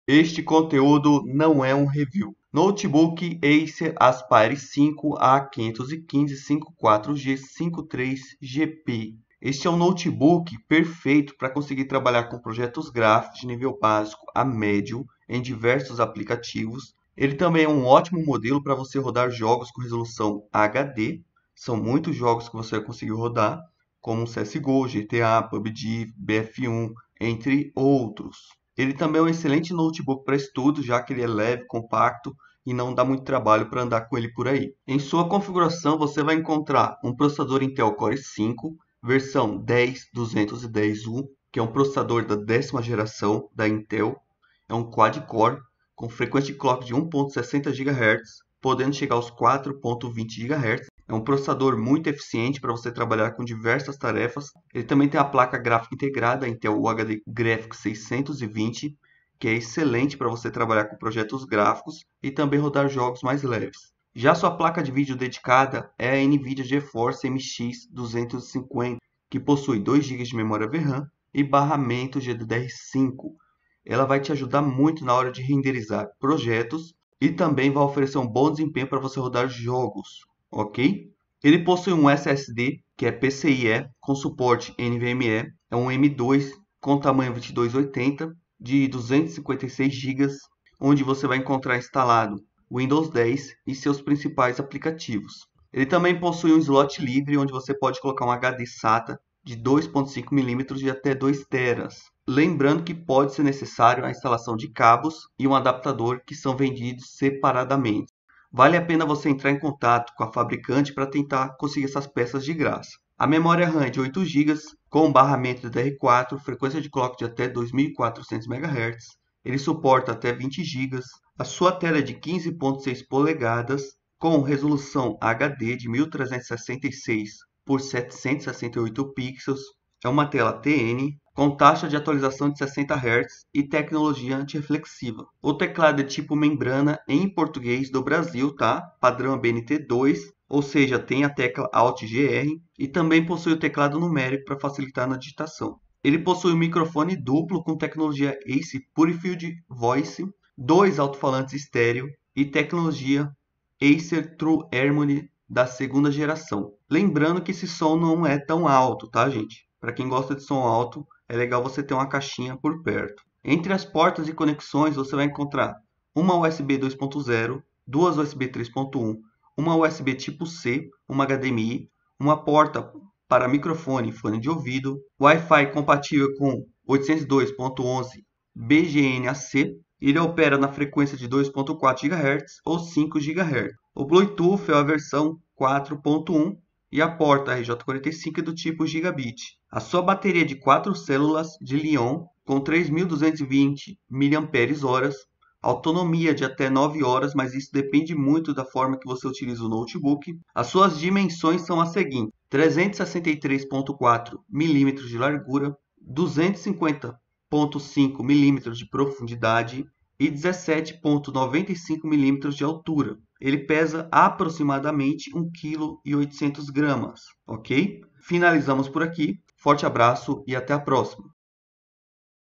Notebook para comprar em janeiro de 2021 Notebook Acer Aspire 5 A515-54G-53GP
Assine Amazon Prime - - visite - - Notebook para comprar em janeiro de 2021 Notebook Acer Aspire 5 A515-54G-53GP

 Este é um Notebook intermediário que pode trabalhar com COREL DRAW, AUTOCAD, REVIT, SOLID WORKS, PHOTOSHOP, PREMIERE e muitos outros. Os projetos realizados devem ser de nível médio e dependendo do aplicativo pode ser até de nível alto. Em caso de jogos, saiba que ele vai rodar CSGO, GTA V, BF1, FORTNITE, LOL, THE SIMS e muitos outros. Muitos jogos devem rodar no nível médio ou baixo e os mais leves podem rodar até em gráficos altos.

WhatsApp,  Telegram,  Messenger,  Instagram,  Tik Tok,  Facebook, Rumble,  Odysse,  Twitter,